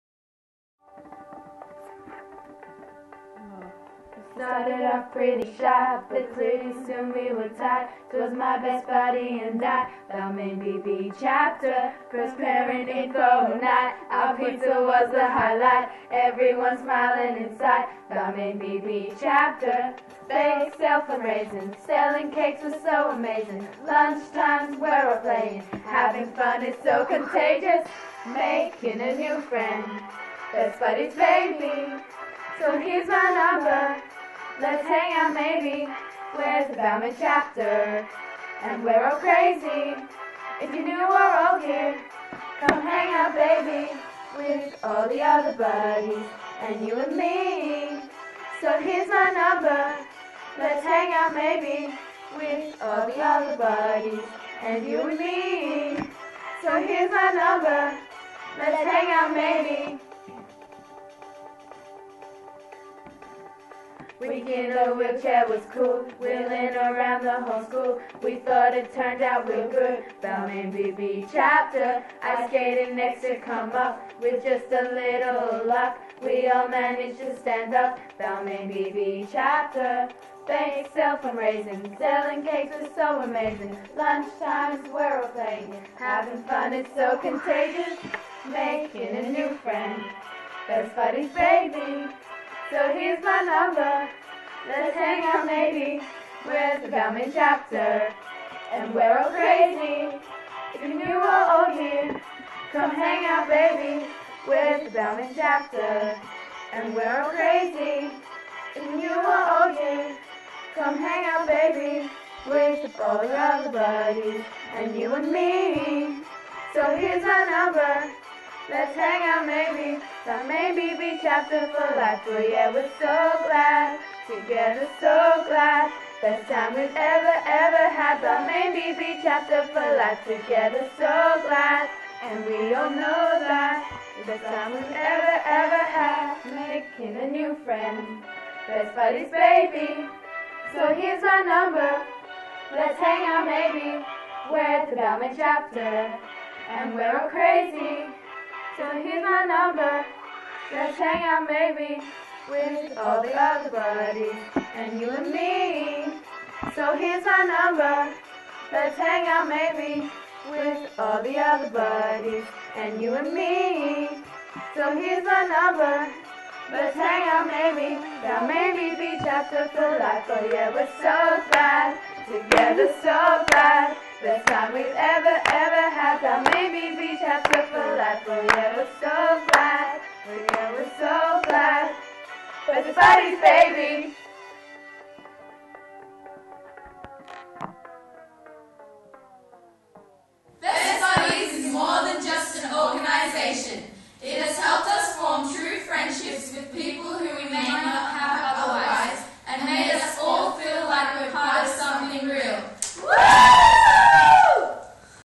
About Us
So this is our new channel as you know. We are more than just an organisation were all well. BEST BUDDIES!!! :D this is a bit about us, a segment of our chapter, a piece in the puzzle, etc. Its just a bit hard to put EVERYTHING into one movie but we hope you enjoy!